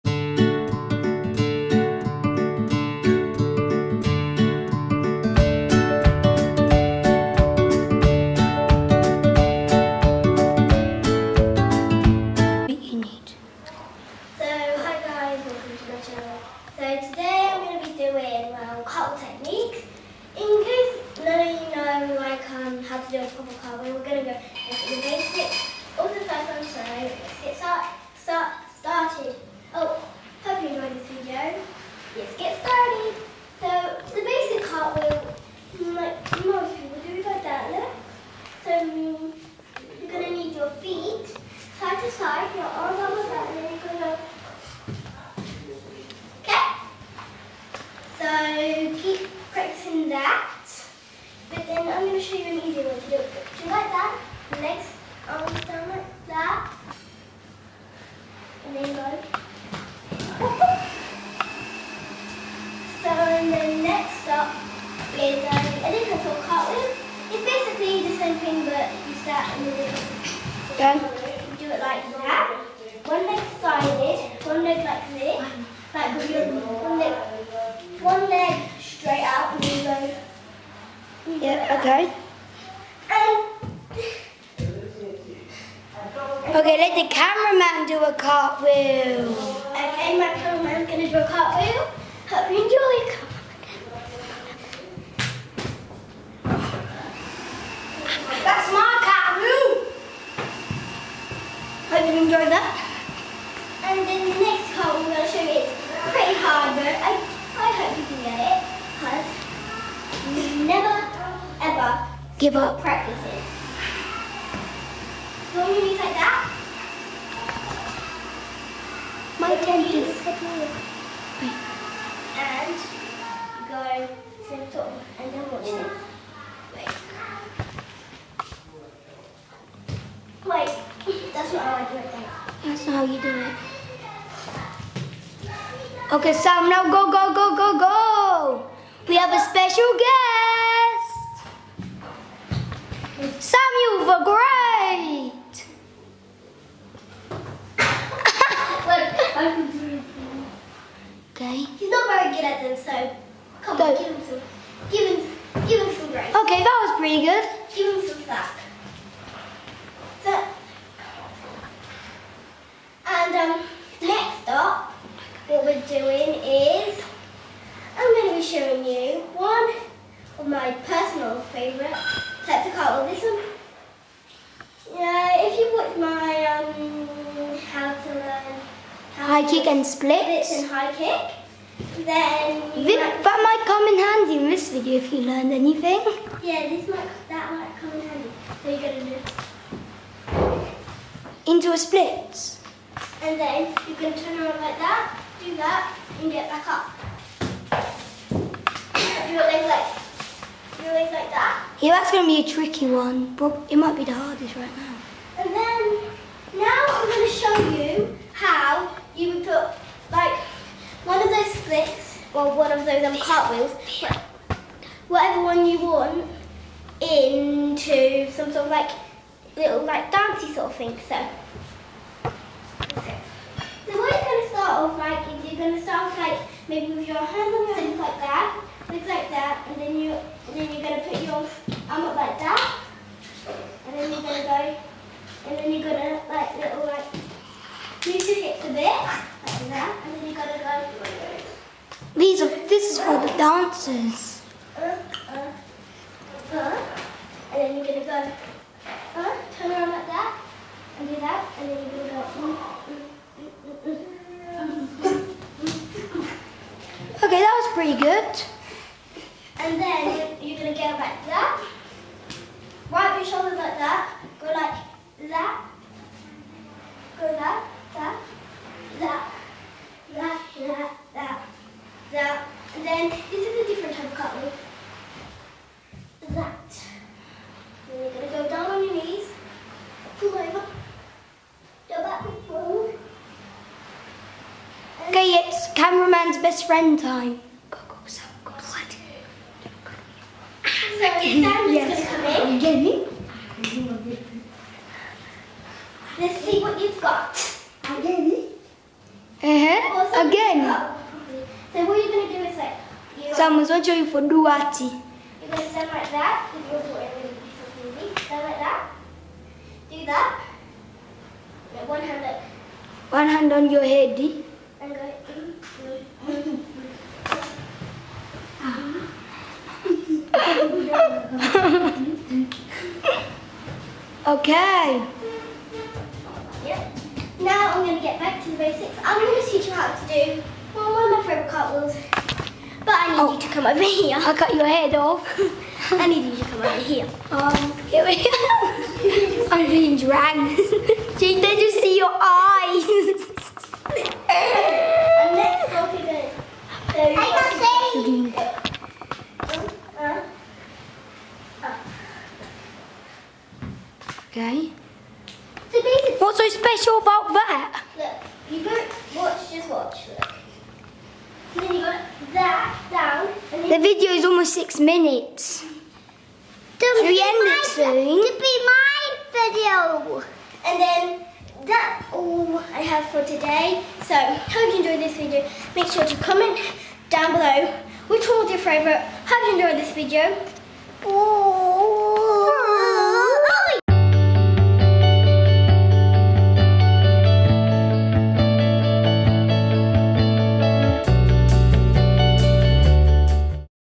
Apphia teaches you some cartwheel technique.
Apphia shows you some of her cartwheel techniques.